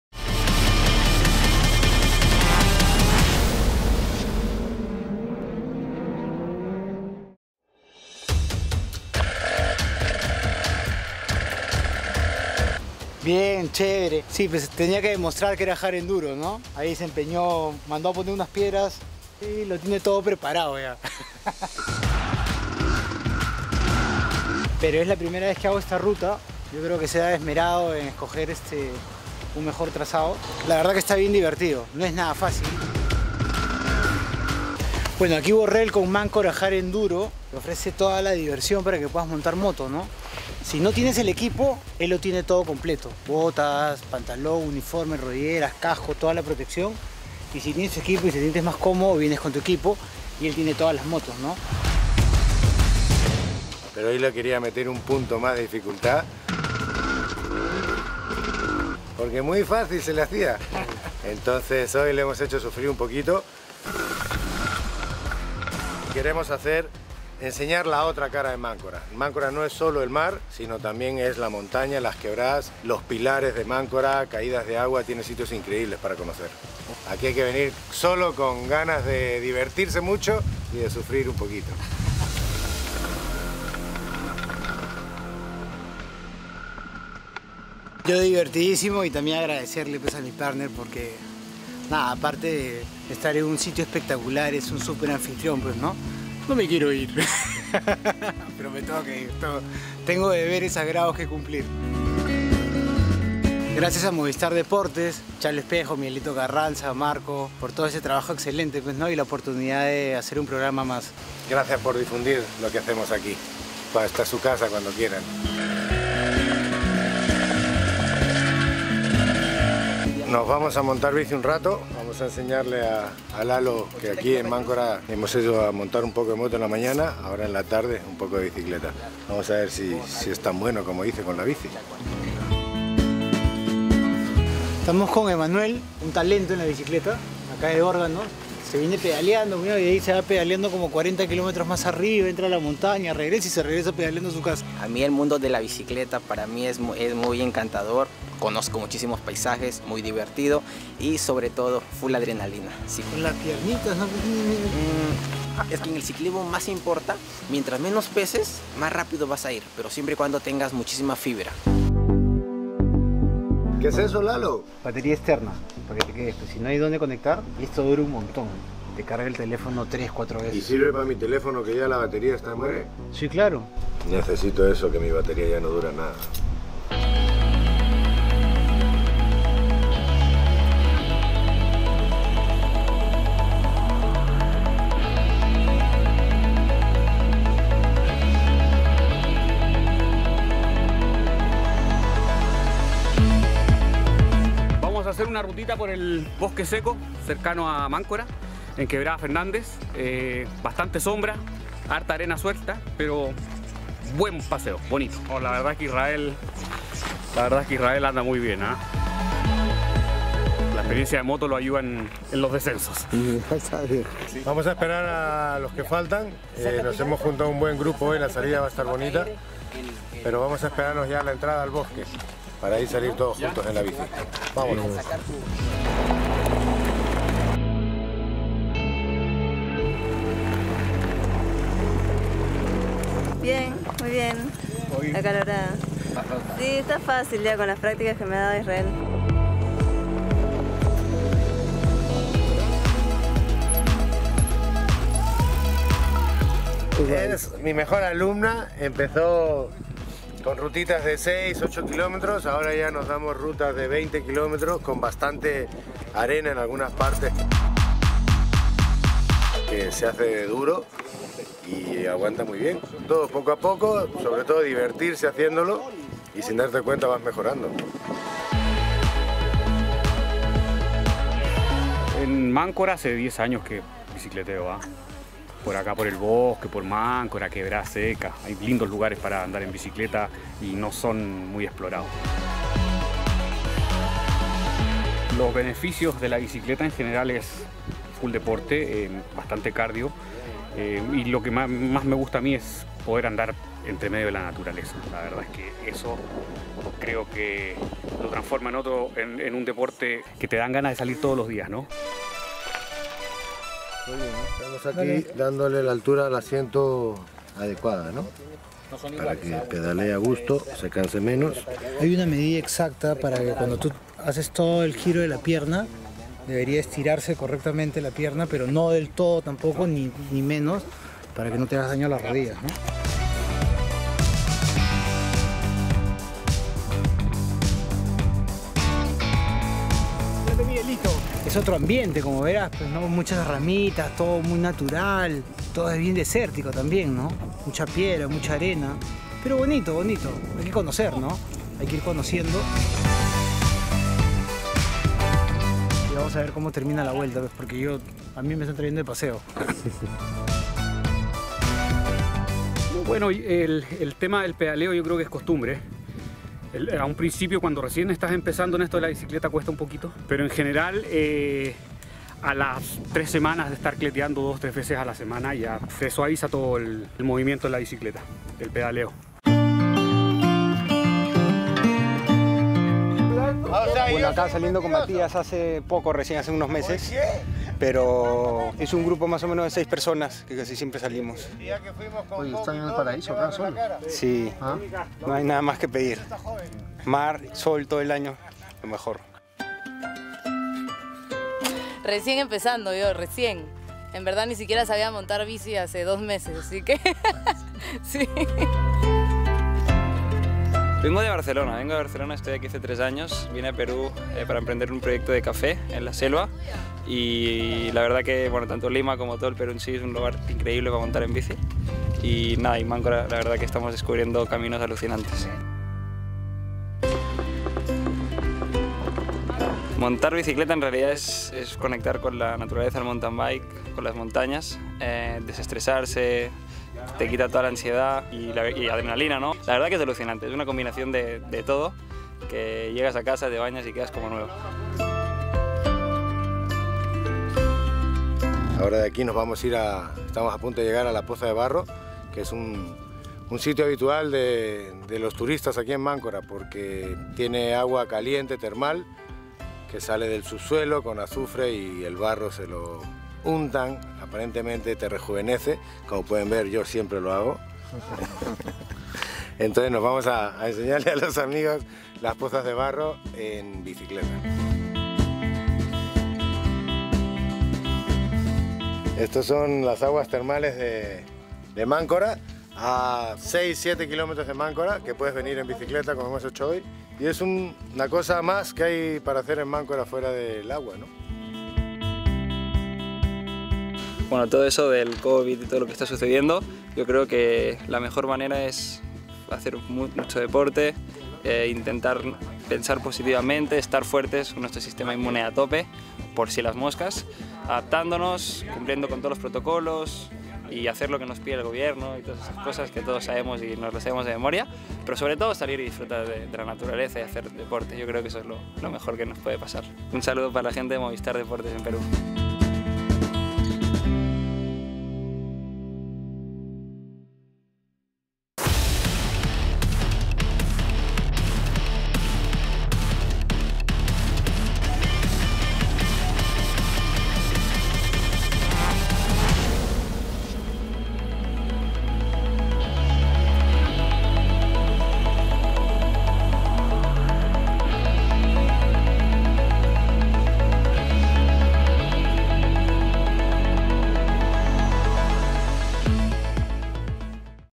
Israel Borrel y Lalo Burga nos muestran Máncora en bicicleta| FULL RUEDAS
Conoce la otra parte de Máncora en bicicleta de montaña, junto a Israel Borrel y Lalo Burga, representantes del Perú en el Rally Dakar